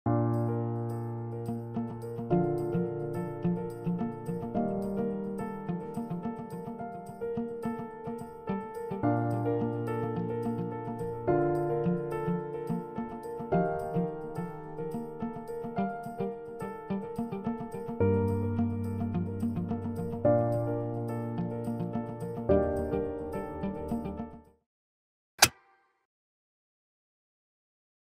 The Frame TV x Gemma Miles | Samsung
Doesn't @_gemmalouisemiles' home just make you want to curl up with a scented candle and a warm drink? The Frame TV is the perfect way to stay cozy and entertained all season long.